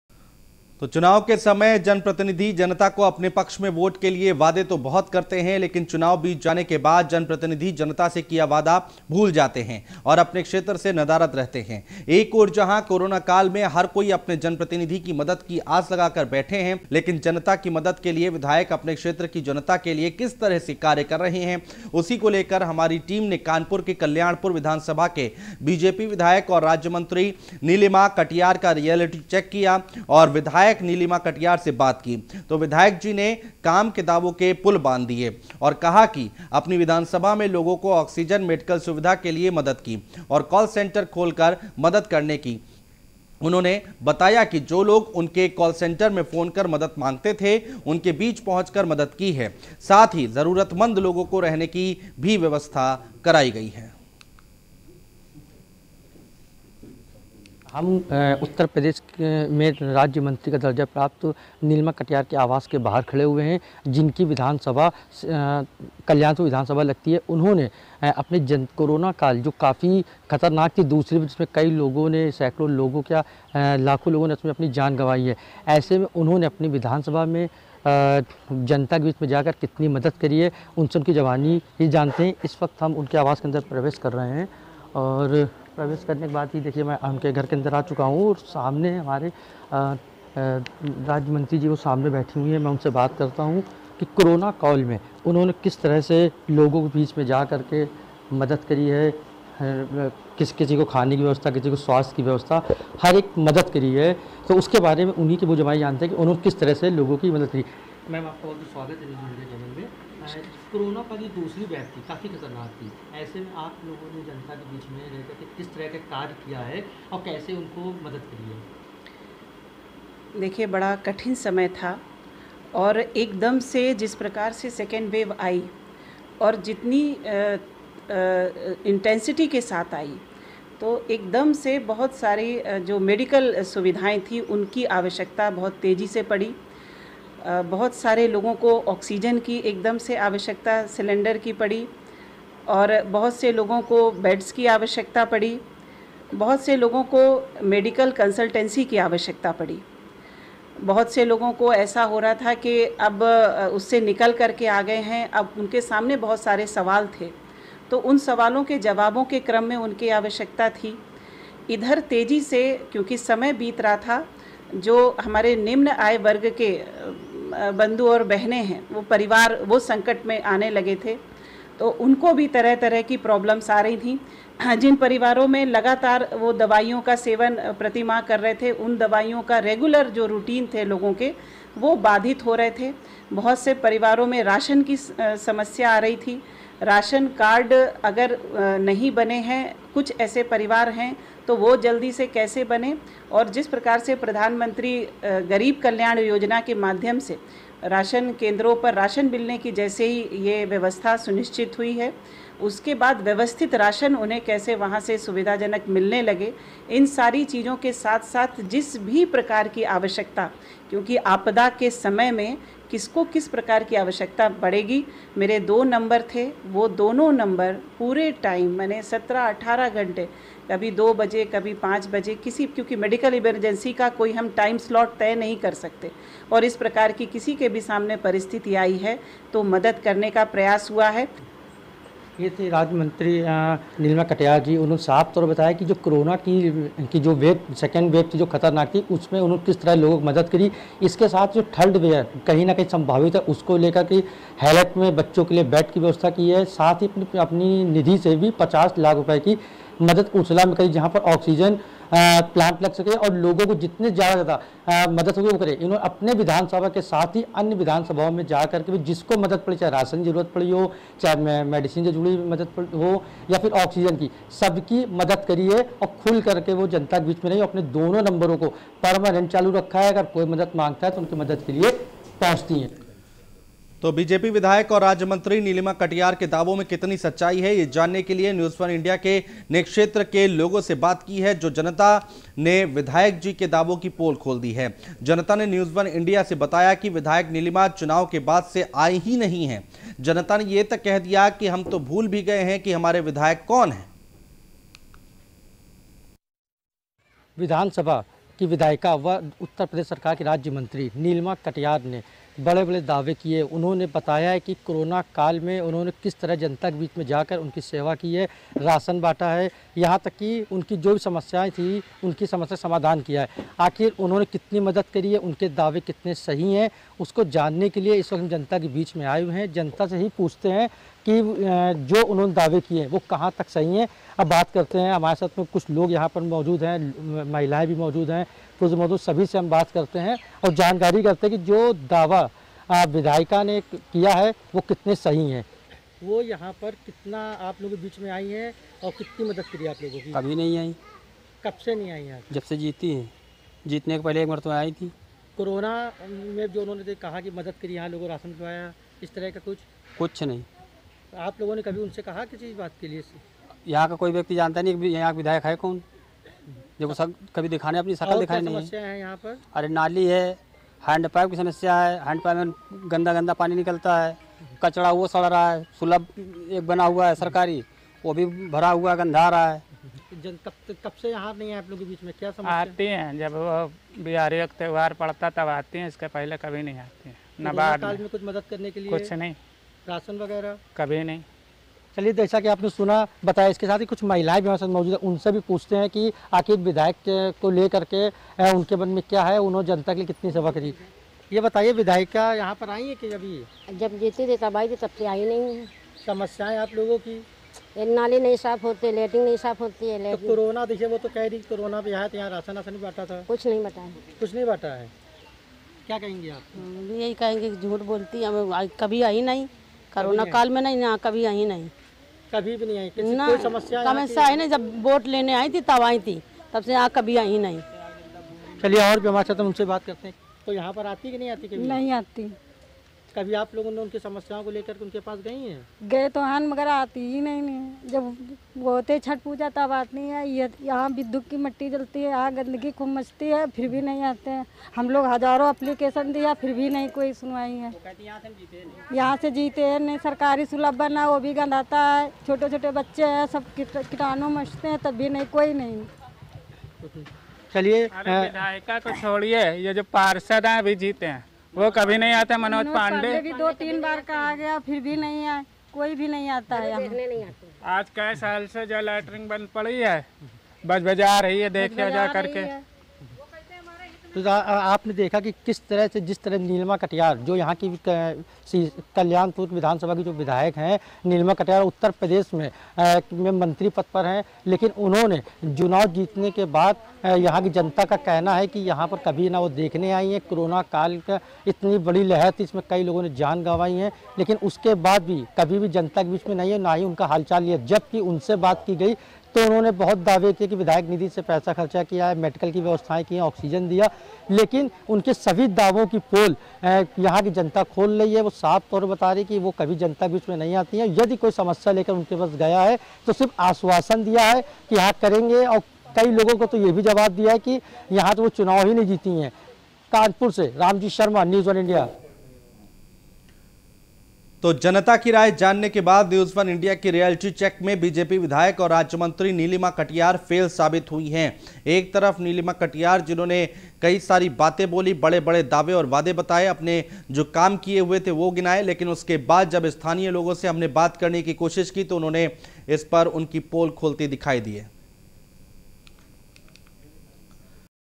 विधायक के दावों को जनता ने नकारा...।।News1india पर देखें Kanpur MLA Neelima Katiyar का रियलिटी चेक
Airtel - 350,
Netvision - 219,
RCN - 403,
UCN-223
Alisha Digital- 285
HDS- 127

We are also available on:
MX Player
JIO TV